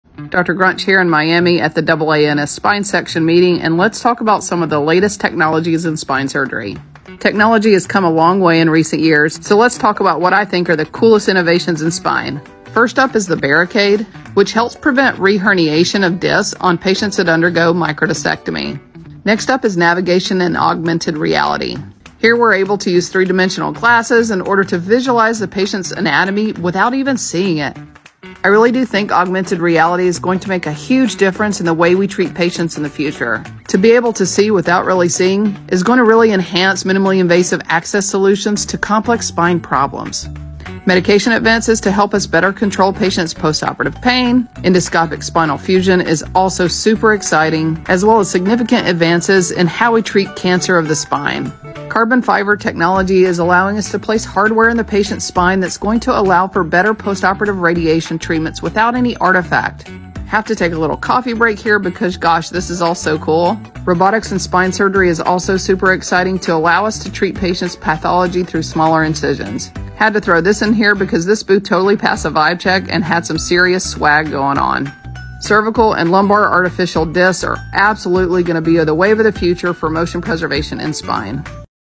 Learn about the latest technology advances in neurosurgical spine surgery with Dr. Betsy Grunch
2023 Spine Summit - See some of the latest technology in neurosurgical spine surgery!